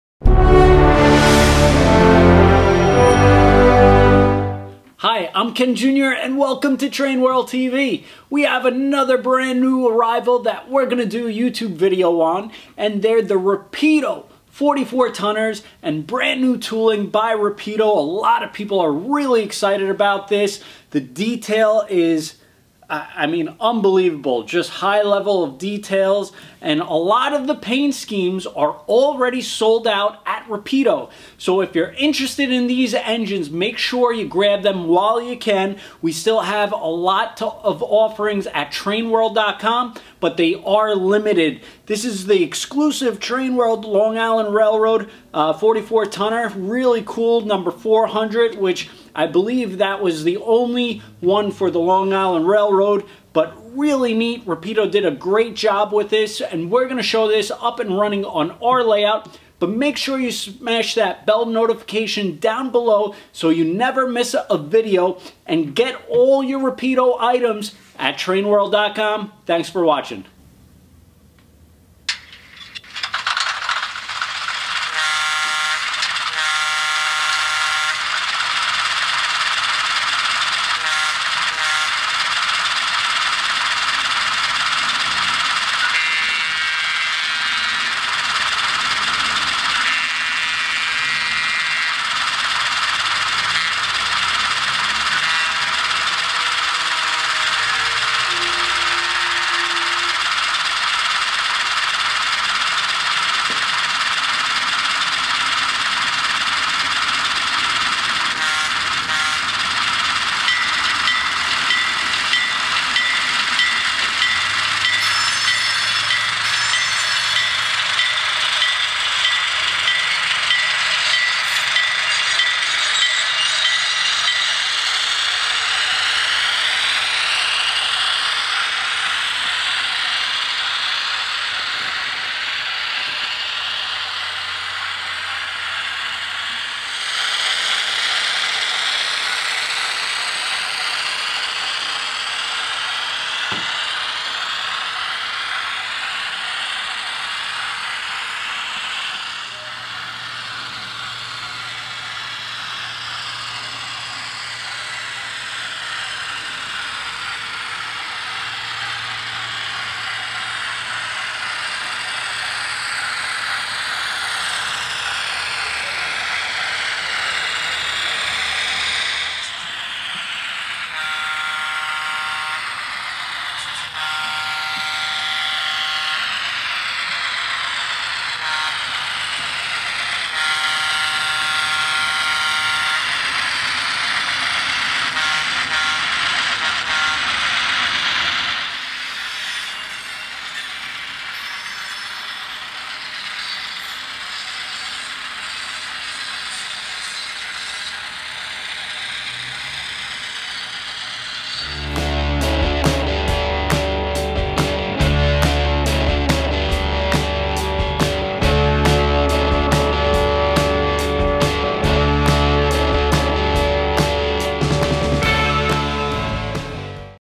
Rapido HO Scale 44 Ton Long Island Railroad
RAPIDO 48531 HO GE 44 TONNER (DC/DCC/SOUND): LONG ISLAND RR - GOODFELLOW: #400

The Rapido Trains HO scale GE 44-Tonner locomotive features: Multiple phases and detail variations (Phase Ic (never offered before in plastic) both with and without lighted hood side number boards, Phase III with positionable top hood hatches, Phase IV), Heavy, die-cast chassis and fuel tank, Correct fuel tank skirting in two styles, Separately-applied door latches, Full cab interior, Photo-etched cab steps, Smooth running, strong coreless motor with dual flywheels, DC models are DCC ready with NEXT-18 decoder socket and speakers, DCC models are sound equipped with ESU decoders and sounds and include our own “Mo-Power” power capacitor